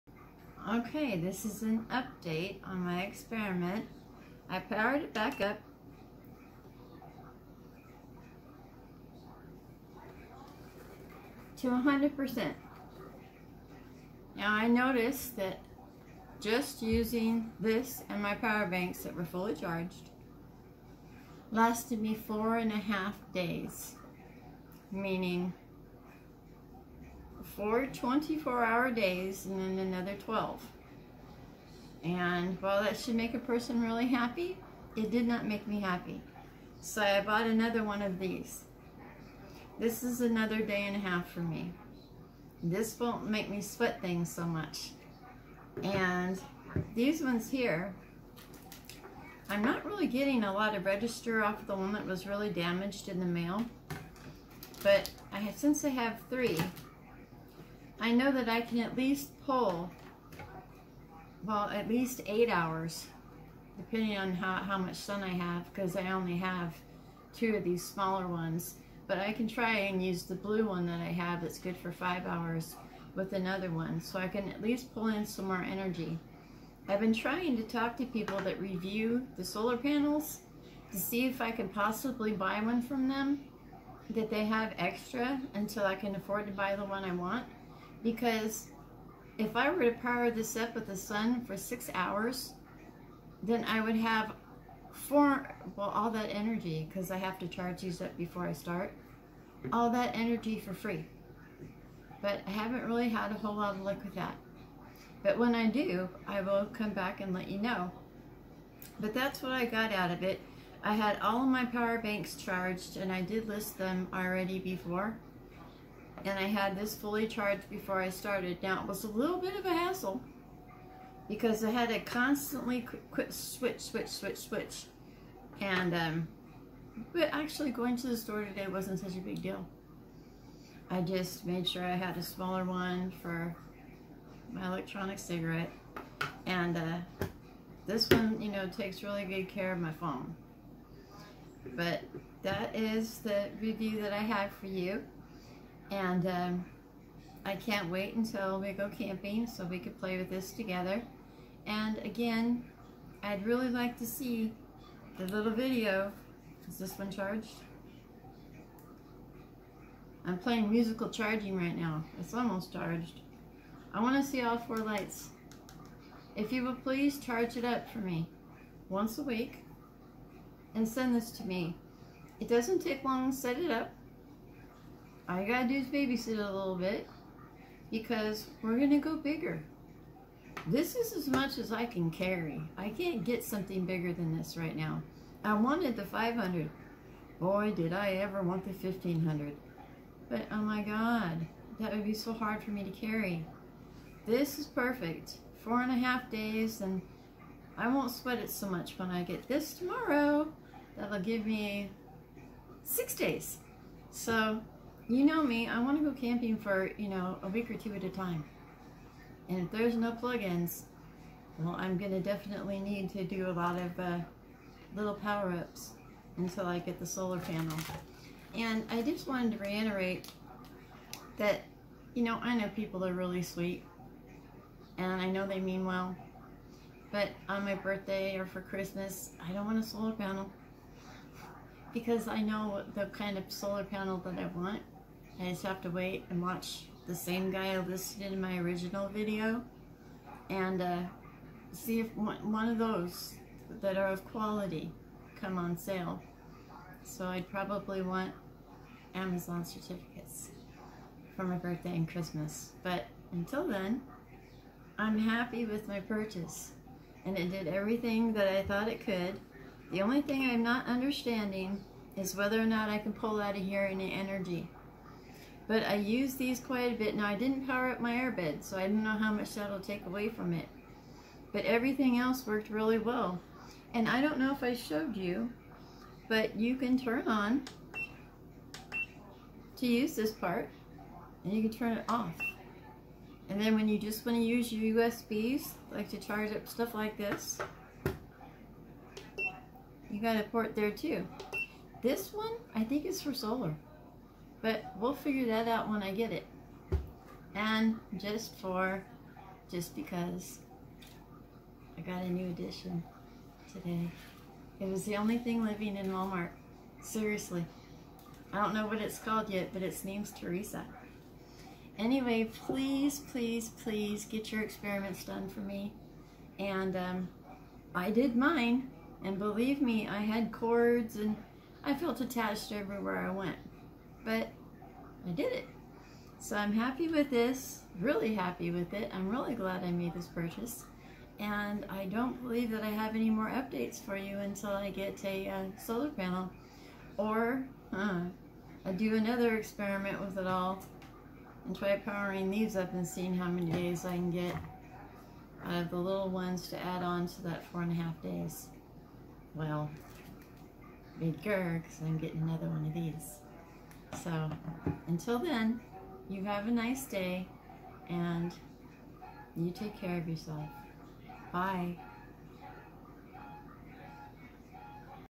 07/08/21 Update GoLabs Power Generator - total hours of use using power banks with generator
GoLabs 300w Power/Solar generator update. Using my power banks listed in my first video, fully charged to start, I was able to see how many days of power I'd have. I'll admit, I bought another one of my big Anker batteries to extend my time 1 1/2 days, as this experiment was progressing. I'll do an update on that as well as using the smaller 6v solar panels to power up the smaller 4 hour banks to extend "days out time"